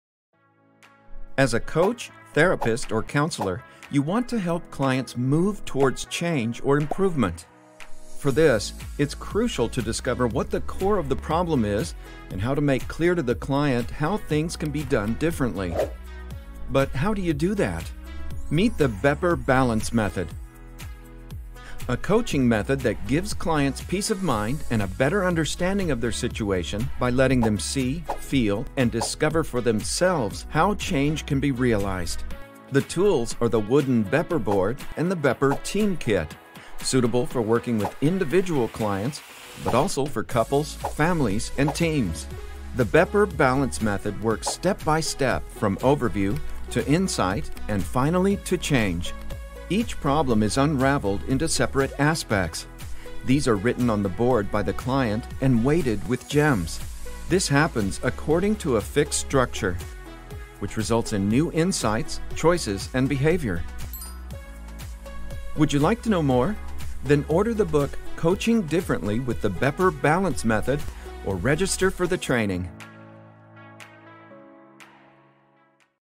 Bepper Balance Coaching Method, explainer ENG
This short animation is About the Bepper Balance Coaching Method and tools.